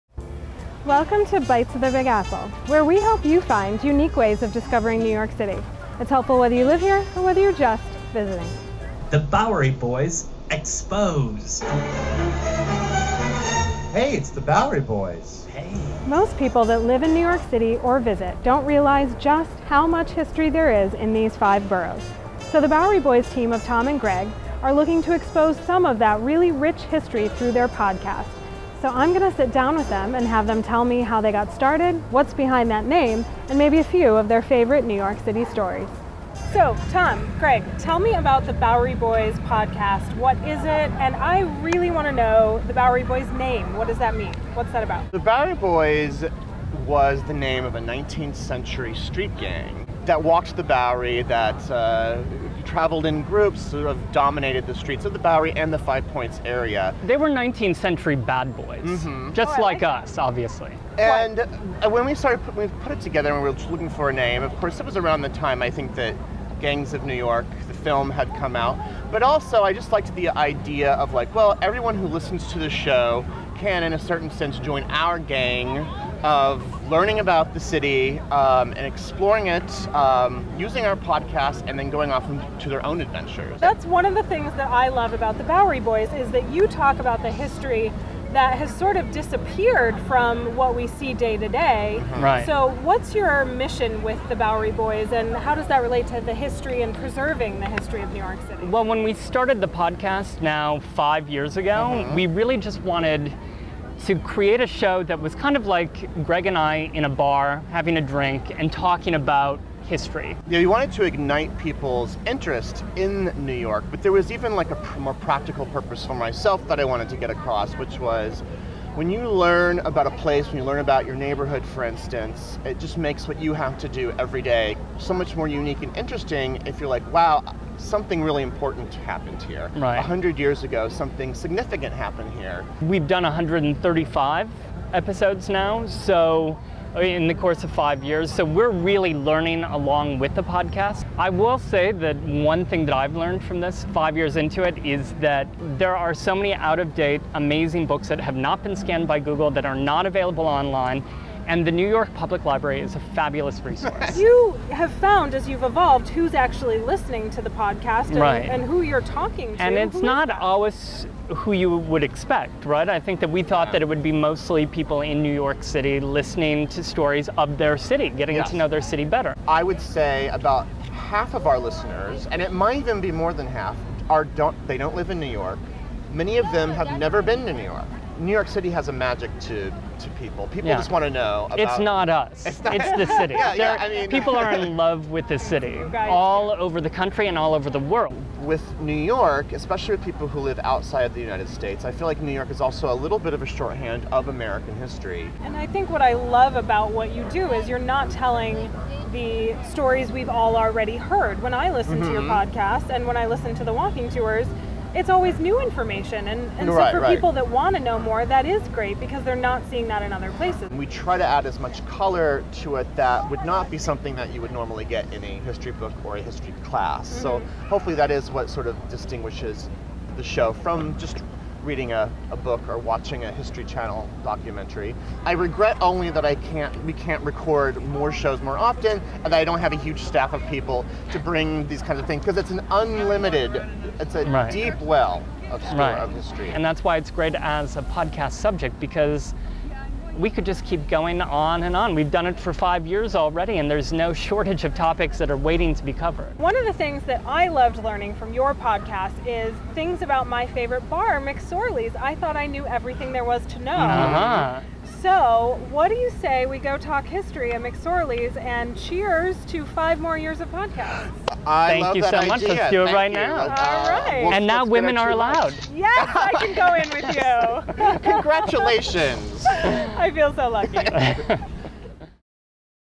NYC History with The Bowery Boys
Meet Tom Meyers and Greg Young - the faces behind the voices of the popular NYC podcast 'The Bowery Boys'. For 5+ years, Tom & Greg have been sharing the rich, colorful history of NYC and they share with us the story behind their name and what inspired them to get started.

as well as on iTunes.